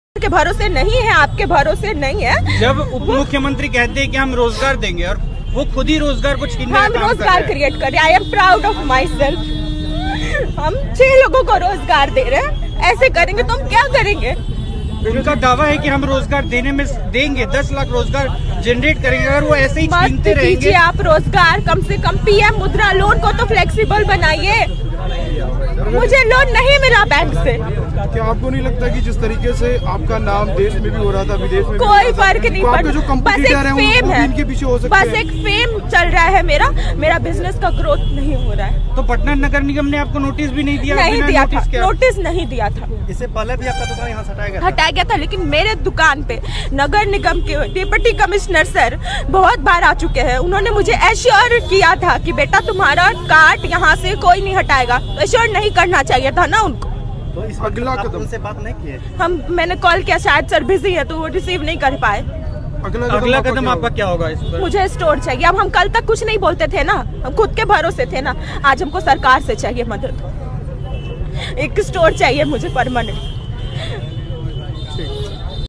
Graduate Chaibali का स्टोल पटना नगर निगम ने तोड़ दिया.😭😭😭 रो रो कर हुआ बुरा हाल.@priyankaGupta @पटना
nagar nigam me a stole toda😭😭😭😭😭😭😭😭😭😭😭😭😭😭😭😭😭
gusse me Priyanka Gupta sarka se mang ki new stole.
Priyanka Gupta chaibali
Priyanka Gupta graduate chaibali
patna graduate Chaibali
Priyanka Gupta
Priyanka Gupta Chaibali
patna Chaibali